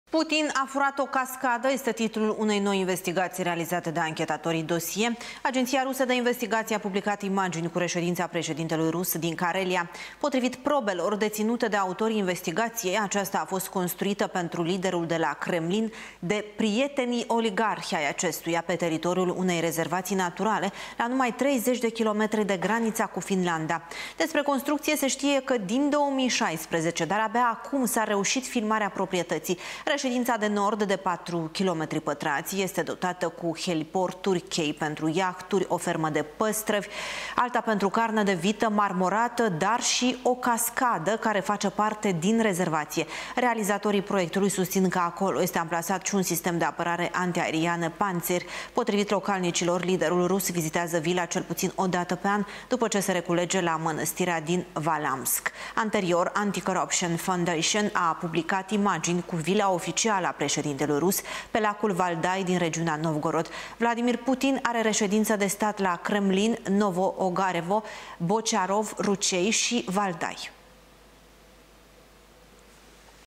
Investigație: Putin ar avea o reședință secretă în Karelia
„Putin a furat o cascadă!" - este titlul unei noi investigaţii realizate de anchetatorii «Досье». Agenţia rusă de investigaţii publică imaginii cu reședința secretă a preşedintelui rus din Karelia. Potrivit probelor deţinute de autorii investigaţiei, aceasta a fost construită pentru liderul de la Kremlin de prietenii-oligarhi ai acestuia pe teritoriul unei rezervații naturale, la numai 30 de km de graniţa cu Finlanda.